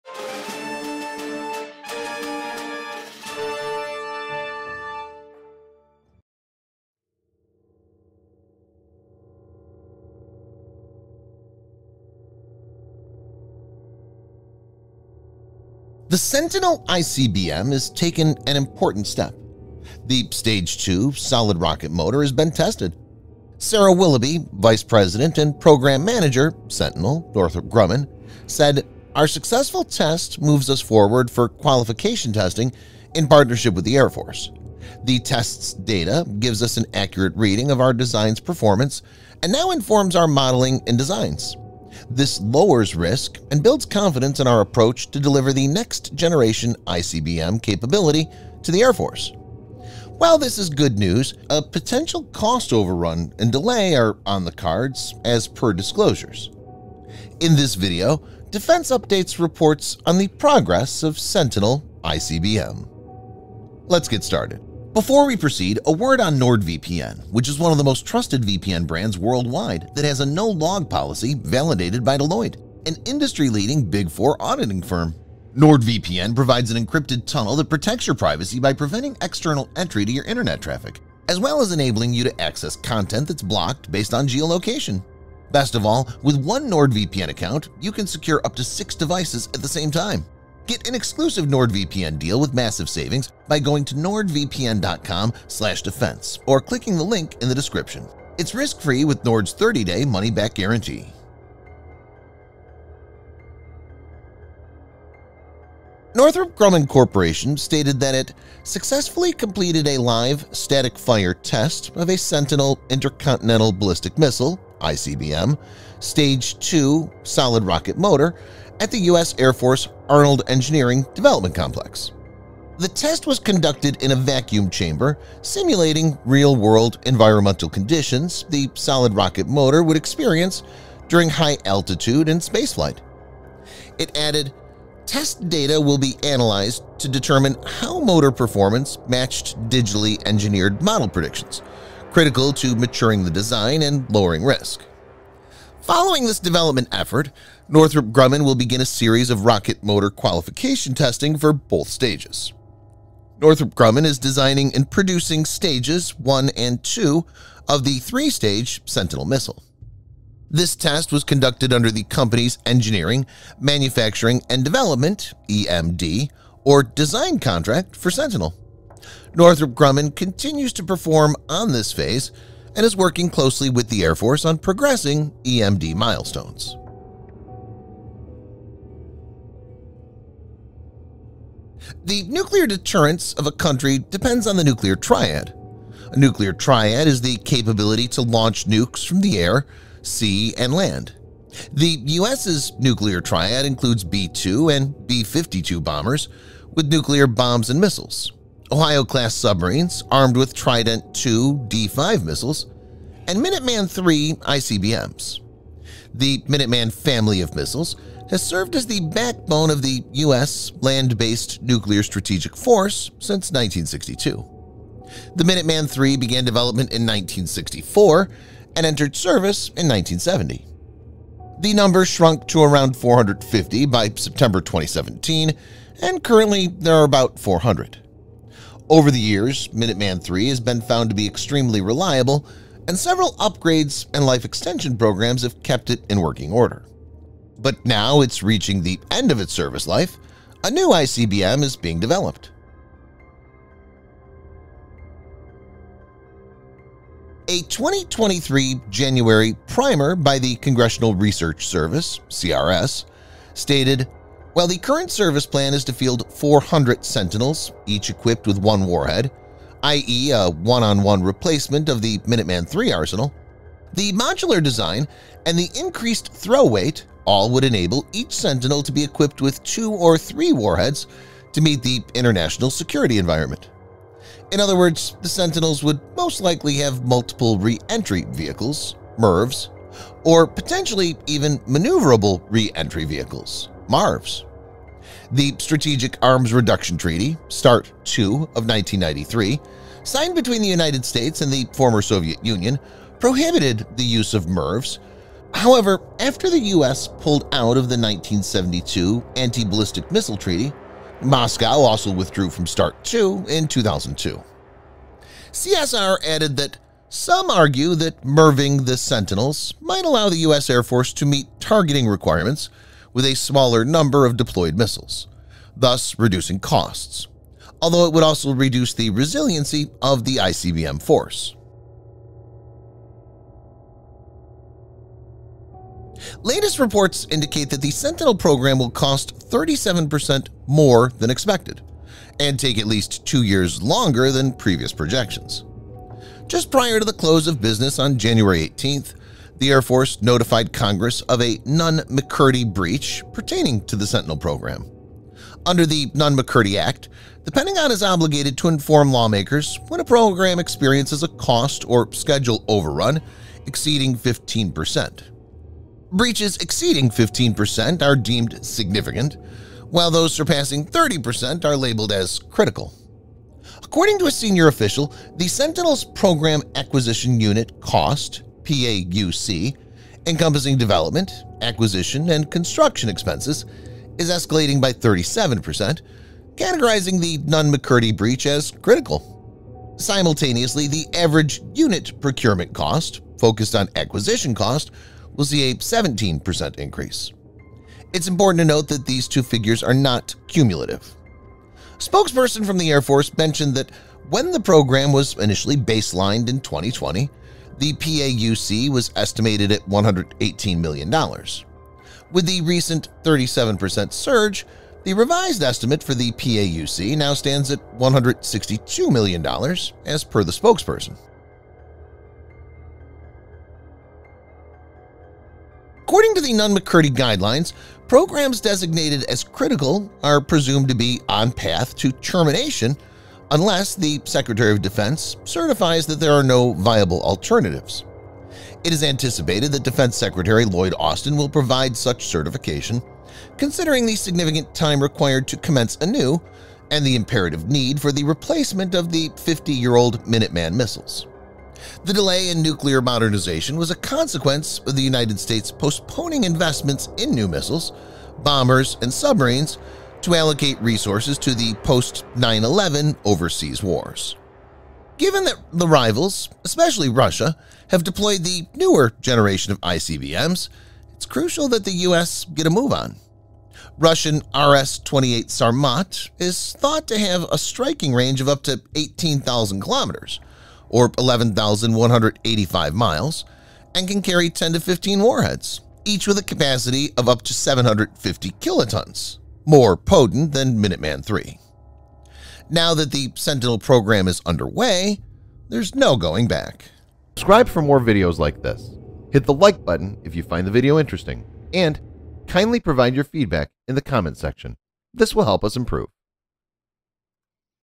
Replacement for MinutemanIII - Progress of SentinelICBM !
The Sentinel ICBM has taken an important step.
The stage-two solid rocket motor has been tested.

The test’s data gives us an accurate reading of our design’s performance and now informs our modeling and designs. This lowers risk and builds confidence in our approach to deliver the next-generation ICBM capability to the Air Force.”

While this is good news, a potential cost overrun and delay are on the cards as per disclosures.

In this video, Defense Updates reports on the Progress of Sentinel ICBM

Chapters:
00:11 INTRODUCTION
01:41 Statement From NORTHROP GRUMMAN
02:56 CRITICAL NEED
04:02 WORTHY REPLACEMENT
05:21 COST OVERRUN & DELAY
07:14 ANALYSIS